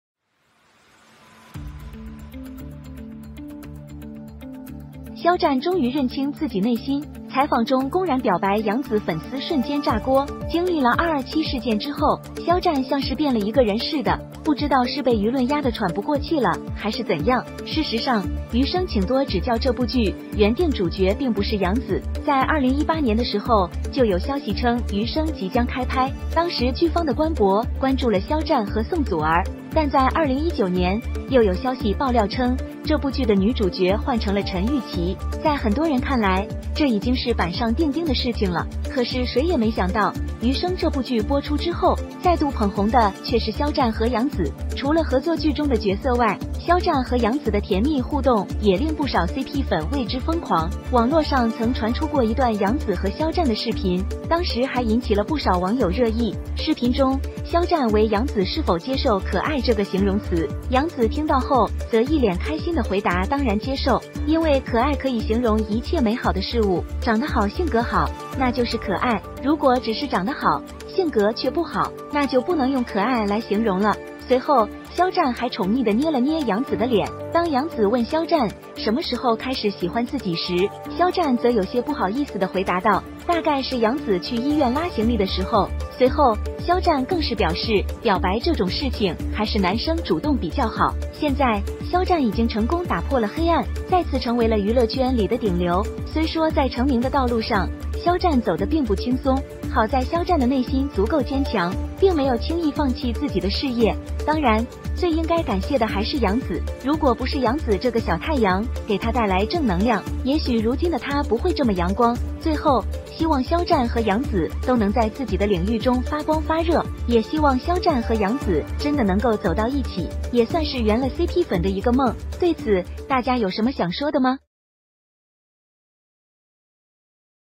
Bazaar Charity Night Fight for C position? Yang Ziqianxi stepped aside one after another
The annual Bazaar Charity Gala can be described as lively, many stars have appeared on the party, Bazaar Charity Night is always a star-studded night, the stars on the scene have put the best side of the show, and in the annual Bazaar Charity Night party, the most striking than the group of stars on the red carpet, their shape, clothing, clothing and other group photos are always a bright spot, from the annual Bazaar The charity party group photo, from time to time will happen all sorts of accidents, especially the most popular C position of the fight, always trigger more netizens hot debate. All know that year Zhang Shaohan accidentally accounted for the C position, and was scolded online countless scolded to death, simply incomprehensible, and this thing continues to ferment in the future, most stars have avoided the special C position, the original stage stood on the group photo, but very famous circle of entertainment, stars is accidentally stood in the C position, but also will be scared to the dogs, simply simply do not compete, but also to bypass it, more The stars simply do not dare to stand C position, and even will be more than C position to the two sides of the station, which can understand the mood of the stars, after all, the stars C position really do not have to compete, a balsa charity party on the final group photo, Yang Zi, YiYiQianXi have to stand aside, LeiJiaYin and XiaoZhong is scared out of the expression bag, simply let people can not stand to see.   YiYingQianXi has paid a lot of things over the years, and finally considered to be a popular household name idol, don't lose heart, it's all on the way to come, you just have to work hard to do your own stuff, and in the face of Bazaar Charity Night, the stars have to the stage for a group photo, netizens surprisingly found Yang Zi and YiYingQianXi, the two popular idols are not competing for the C position, not only that, the stars on the stage is very much The actual fact is that the actual people are not only the most popular, but also the most popular. The first thing you need to do is to get a good idea of what you are getting into. The last group photo is also a success, many actresses in glamorous clothes in the same frame, is really eye-catching, from which you can see Zhao Liying and Zhou Dongyu, Gou Naza next to each other, in the shooting three hot chat non-stop, while chatting while covering the mouth laugh, in fact, Zhou Dongyu and Zhao Liying have known each other for a long time, the relationship between the two people is still good, but also look forward to the future Bazaar Charity Gala better and better, today's share ends here, what different views on this, welcome in the comments section message discussion.